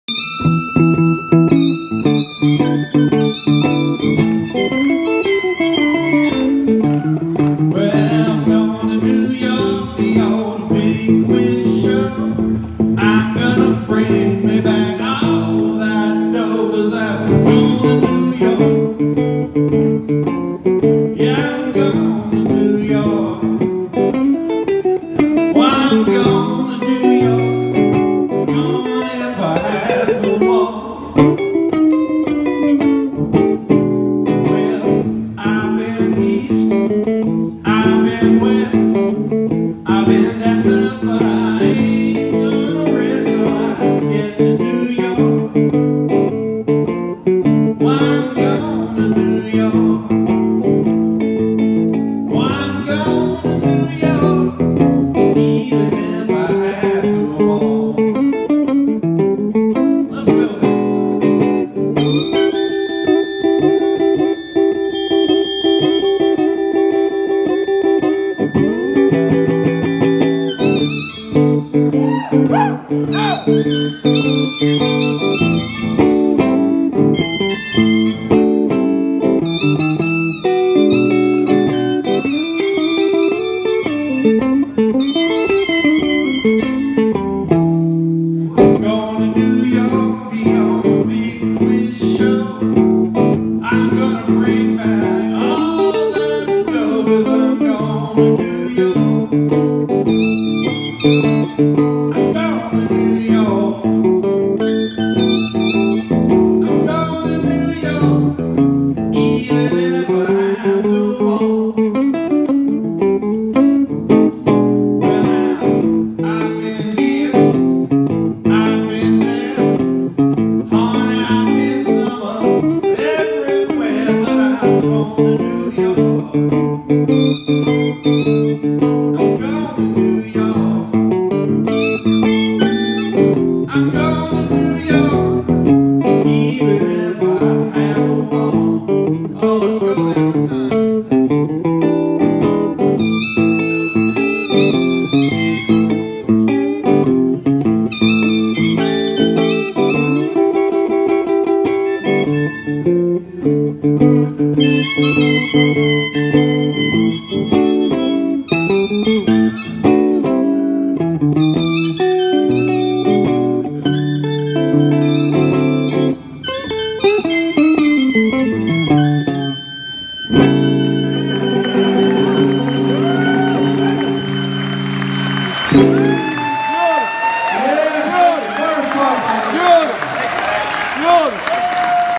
The Great Salt City Blues Concert 3 - Morris Hepcats Tarbell & Tom Townsley - 12-26-18
Palace Theatre, Eastwood, NY - 12-26-18
Morris Hepcats Tarbell - guitar,  Thomas Townsley - harp
As independent artists:
Thanks to Greg Spencer and Blue Wave Records